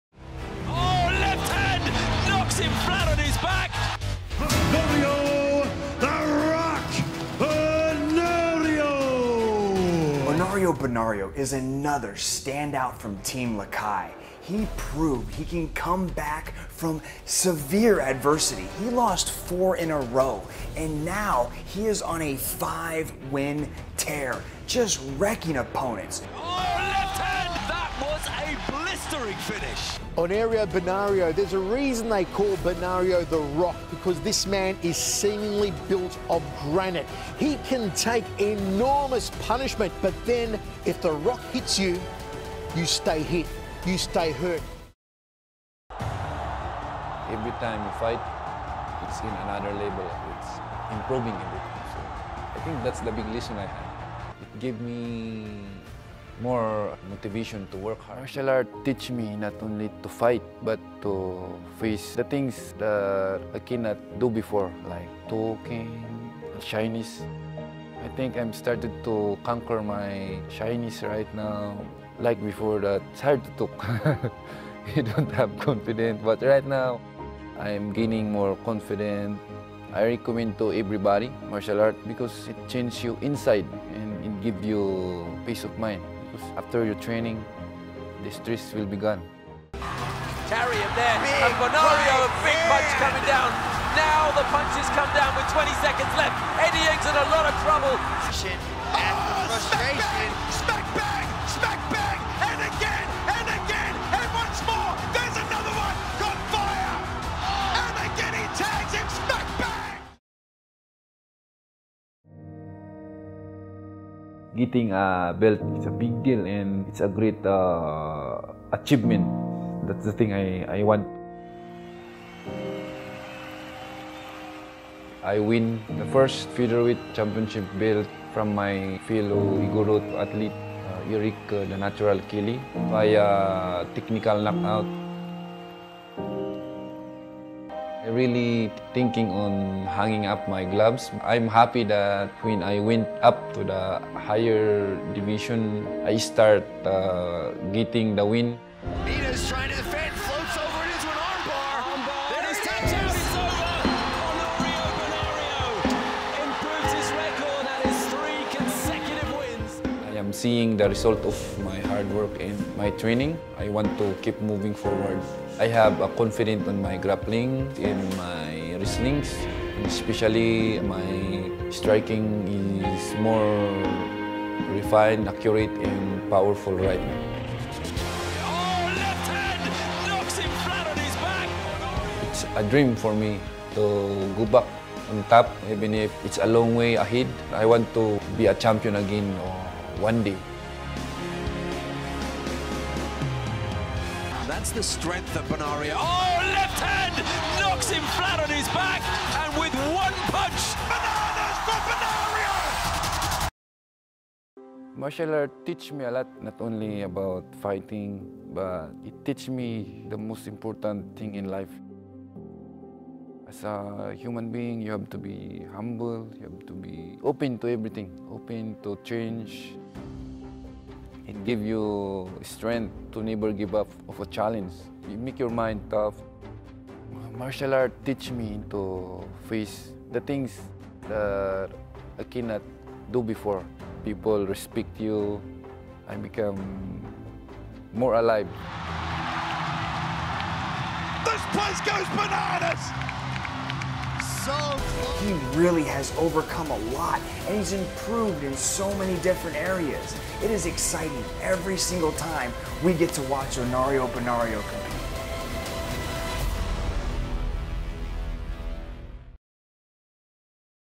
The First Team Lakay Champion in ONE Championship | Honorio Banario
Kilalanin ang pinakaunang Team Lakay fighter naging kampeon sa One Championship, si Honorio "The Rock" Banario.